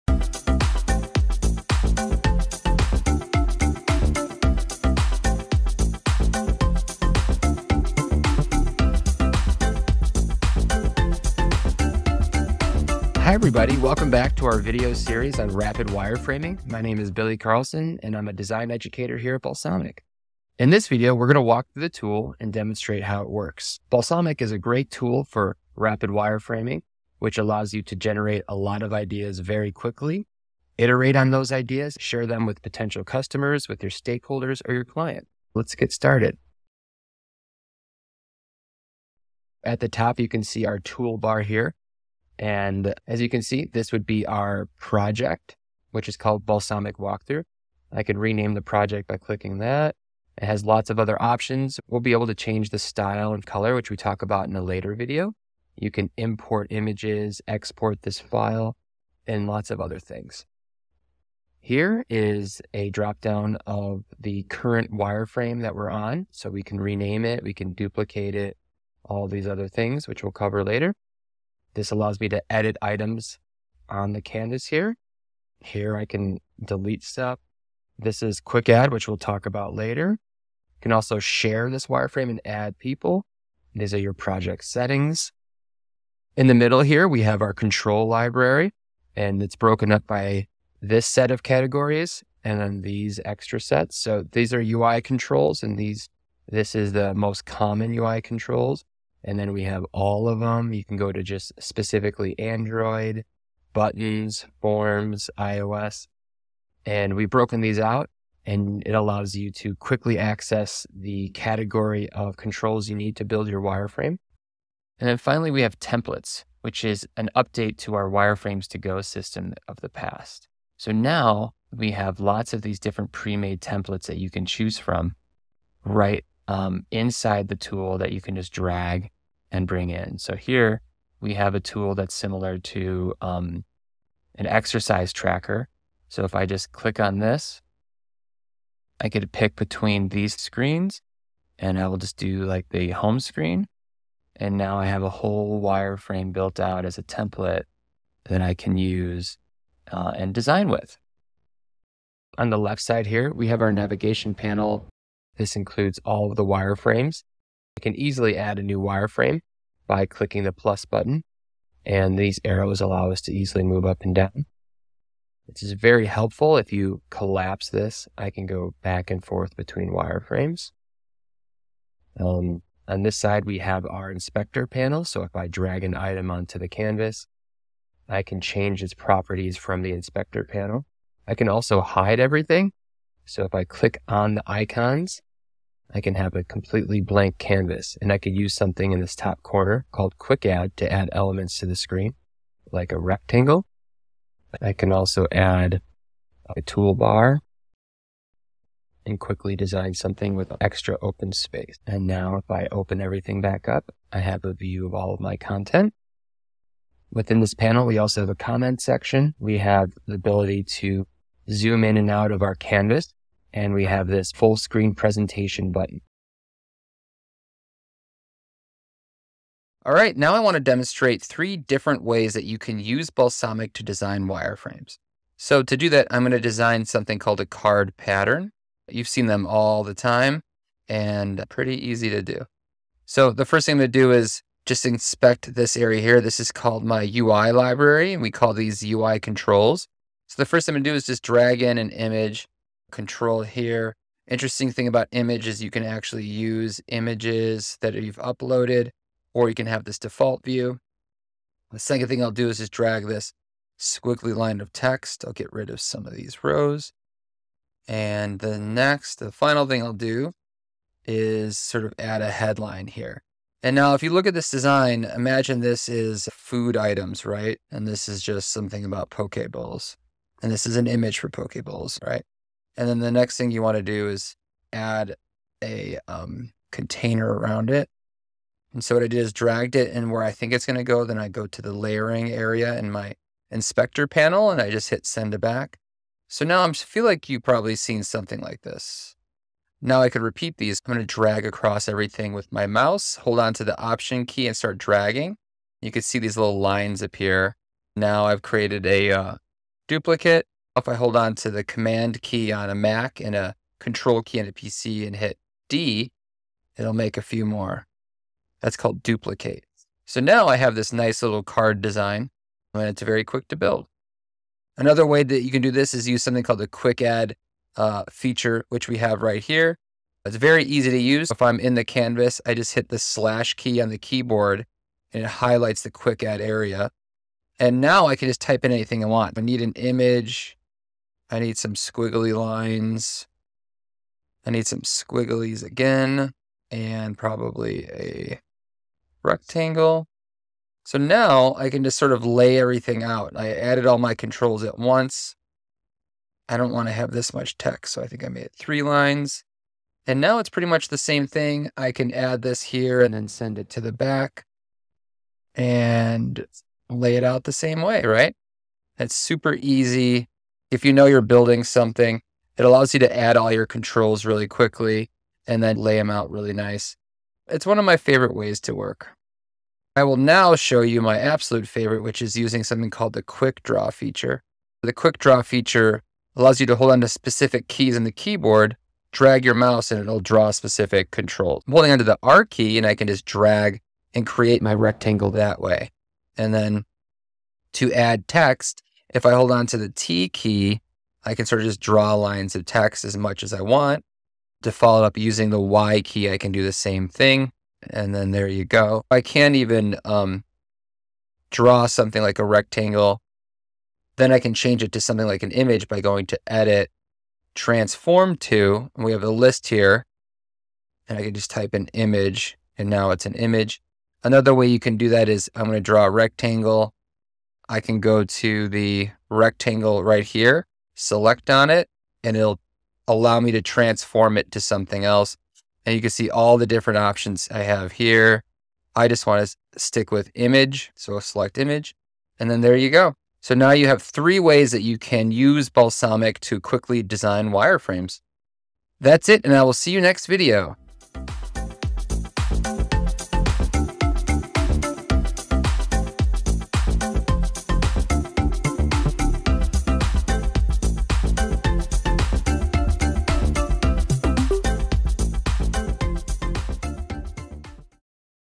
3. Using Balsamiq: key concepts
Watch a walk-through of the Balsamiq Cloud user interface - toolbars, menus, UI control library, inspector panel - and a demonstration of how it works.

Billy will also show you three different ways to create a wireframe in Balsamiq really fast!

Balsamiq Cloud is a rapid wireframing tool that allows you to generate a lot of ideas very quickly, iterate on those ideas, and share them with potential customers, stakeholders, or clients.

👨‍🏫 Meet your instructor, Billy Carlson. Billy is a seasoned UX educator, product design leader, and the author of “Wireframing for Everyone.”